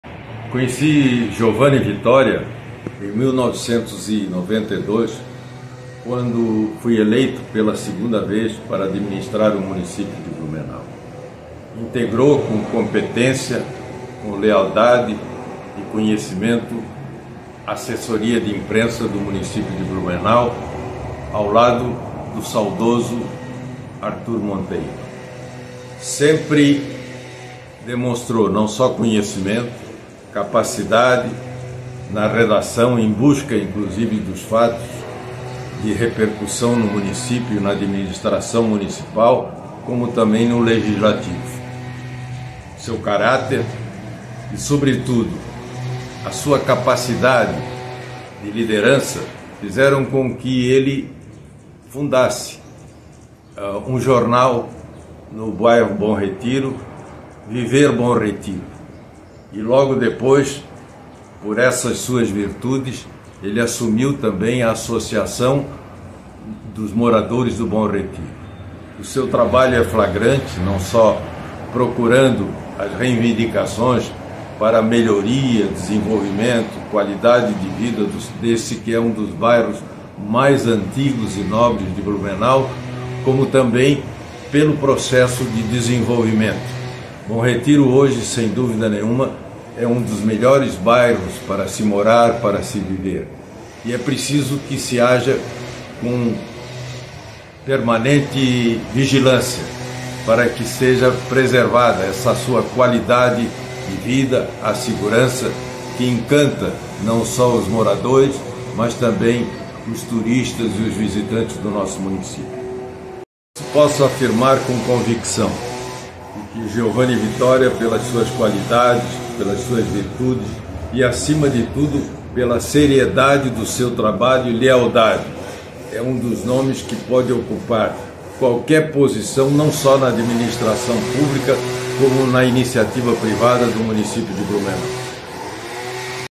Quem conhece minha luta, apoia minha candidatura!
Em 1992, atuei como assessor de imprensa na campanha vitoriosa da dobradinha RENATO VIANNA/Vilson Souza para Prefeitura de Blumenau. Renato Vianna chegou ao Paço Municipal com apoio de 53.392 blumenauenses (44,78% dos votos).

Com sua chegada à Prefeitura, fui honrado com o convite para assumir a gerência de imprensa da então Assessoria de Imprensa – hoje Secretaria de Comunicação –, onde fui chefiado pelo saudoso jornalista Arthur Tadeu Dutra Monteiro.

Foram quatro anos de intensa divulgação das ações e obras da gestão. Destaco algumas que impactaram na cidade: A construção da ETA 3 (Garcia); dos dois terminais de ônibus do Garcia e o início da integração do transporte coletivo; as pavimentações asfálticas do Morro da Cia (Bruno Hering e da Rua Sete de Setembro); a criação de programas sociais para crianças, adolescentes e idosos; a migração do sistema de saúde de Blumenau para gestão semiplena, permitindo que os recursos viessem diretamente para cidade e a implantação dos primeiros programas de saúde da família e o suporte agentes de saúde nos domicílios de famílias carentes.

Ainda em seu primeiro ano de gestão, foi inaugurado o Shopping Neumarkt e dado os primeiros passos para implantação do Parque São Francisco que ao lado do Vale do Bom Retiro é o “Pulmão Verde” da região central.

Fico honrado em saber que puder dar minha contribuição com o desenvolvimento da cidade naquele período e ainda mais honrado em ouvir esse testemunhal do prefeito Renato Vianna. Como ele, gostaria de ir além e chegar à Câmara de Vereadores e seguir avançando nas melhorias que tanto sonhamos para nossa cidade.

Mas o caminho para pavimentar essa conquista depende da união de todos!

Viva o Bom Retiro!
Viva todos os bairros de Blumenau!
E Viva nossa querida Blumenau!

Facebook (Página): giovanivitoriacandidatovereador
X (Twitter): @giovanivitoria
Canal no Youtube: @giovanivitoria